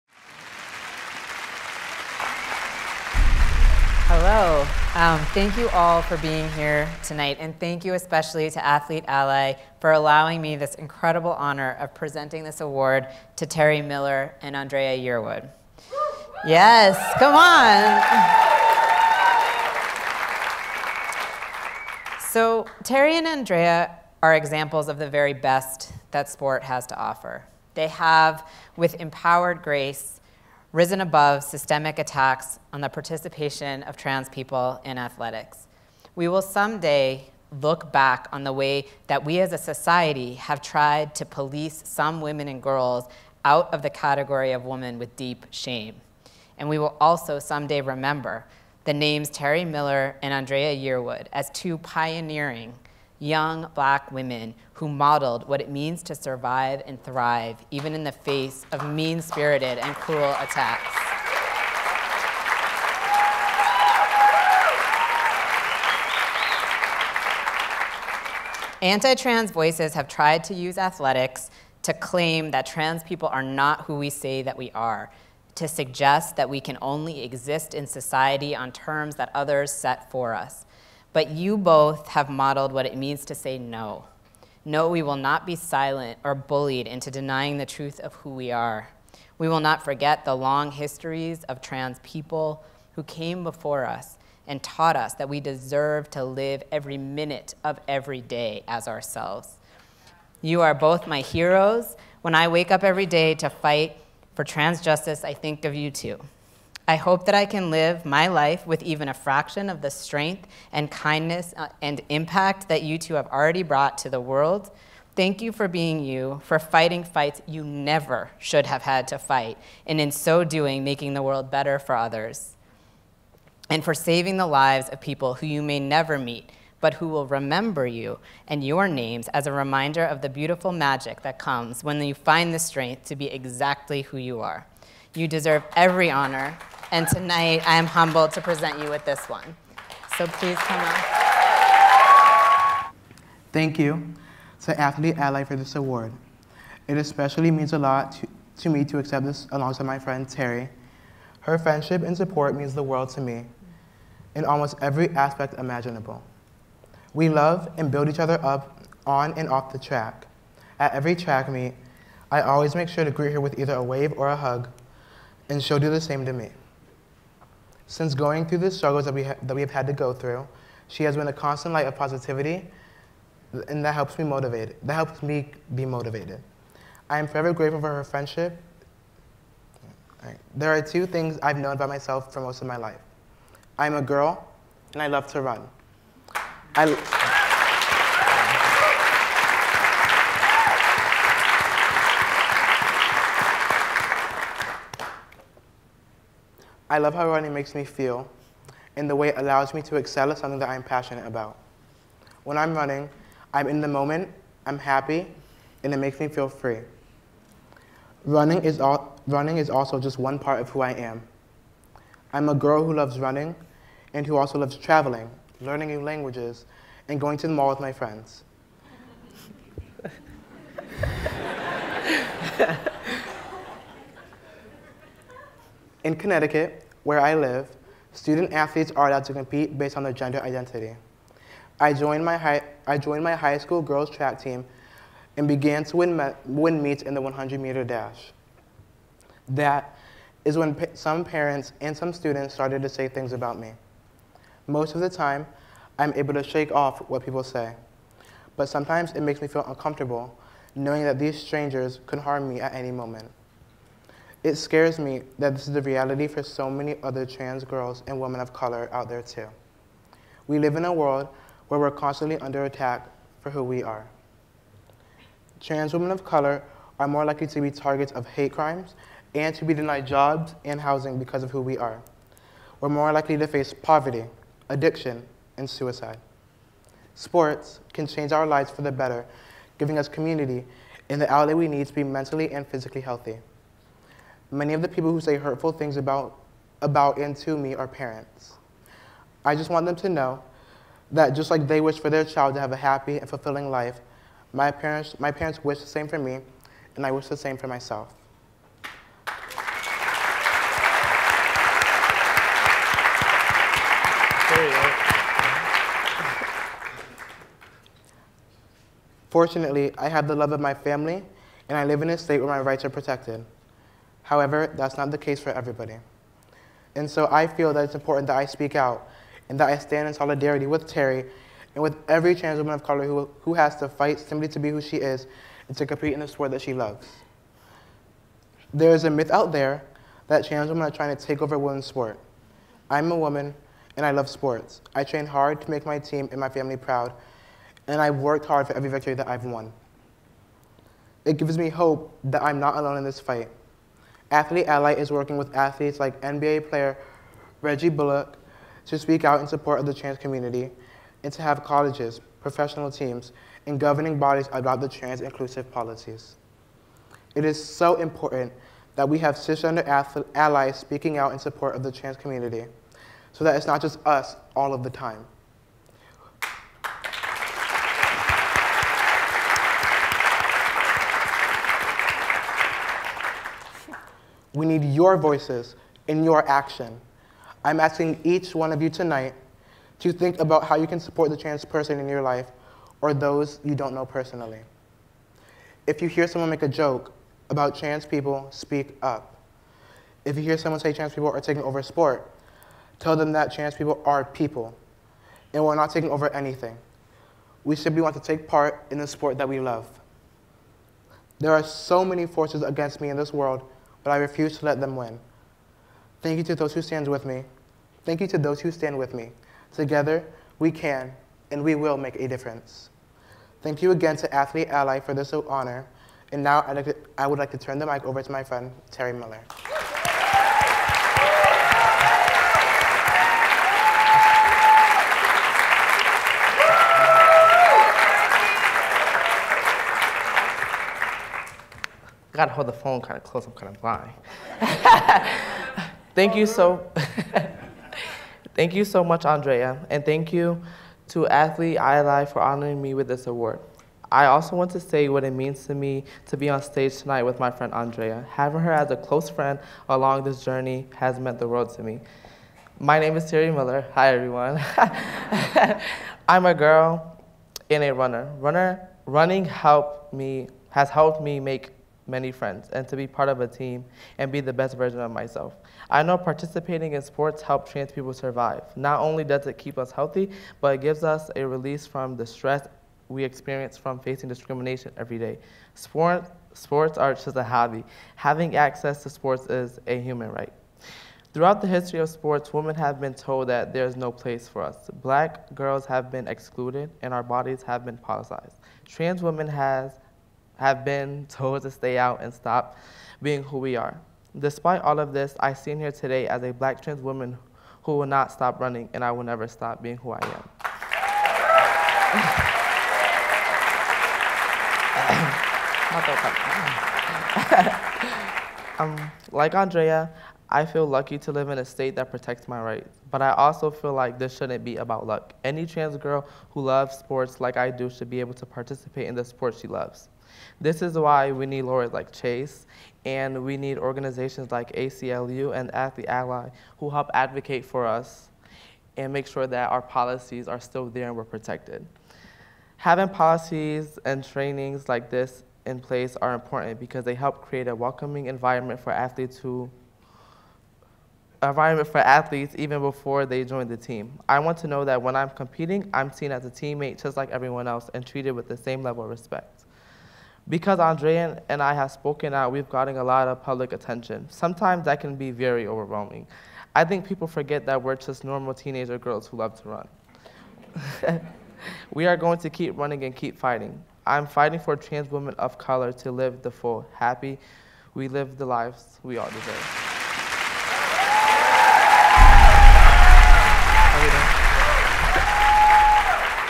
6th Annual Athlete Ally Action Awards: Andraya Yearwood & Terry Miller
Honorees Andraya Yearwood and Terry Miller speak at the 6th Annual Athlete Ally Action Awards.